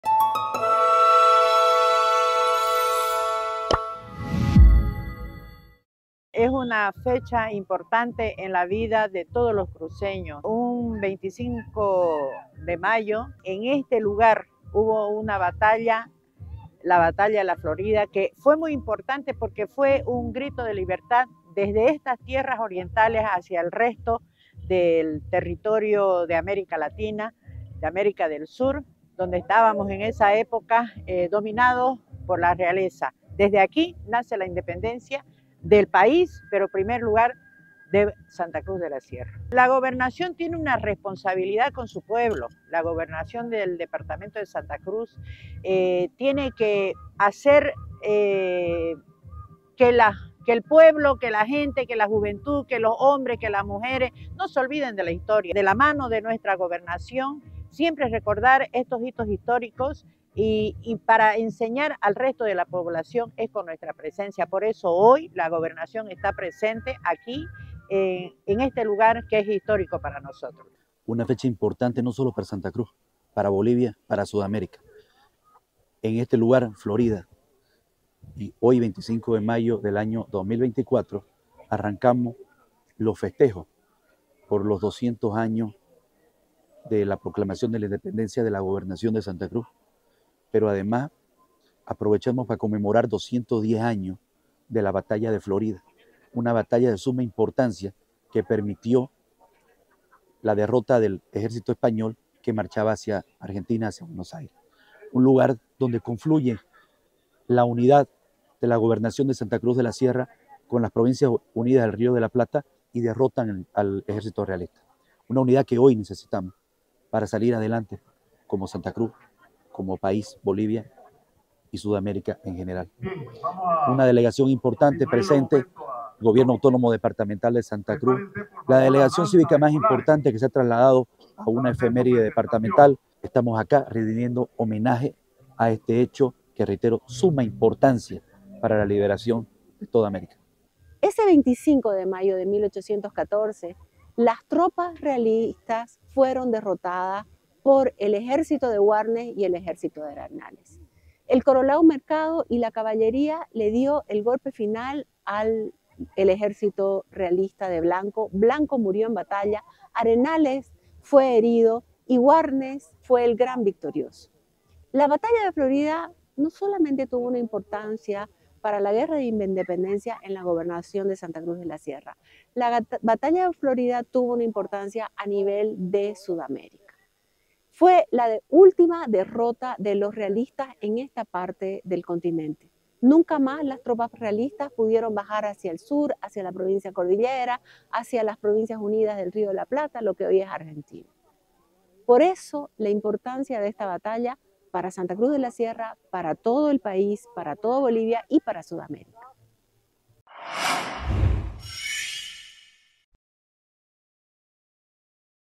210 AÑOS DE LA "BATALLA DE FLORIDA"
La comunidad Florida (Cabezas) fue escenario de un desfile cívico en homenaje a la celebración de los 210 años de la ‘Batalla de Florida’, recordando la gesta librada un 25 de mayo de 1814, en donde los patriotas derrotaron a los realistas, combate que años después culminó con la independencia de Santa Cruz y la fundación de la República de Bolivia.

El acto contó con la presencia de María del Carmen viuda de Prado, directora de Coordinación Territorial de la Gobernación, en representación del gobernador en ejercicio Mario Aguilera, Fernando Larach, presidente del Comité Cívico pro Santa Cruz, Paula Peña, historiadora, concejales de Cabezas, y representantes de otras instituciones que conforman el Comité Impulsor del Consejo Departamental para la Celebración de los 200 años de la Independencia de Santa Cruz.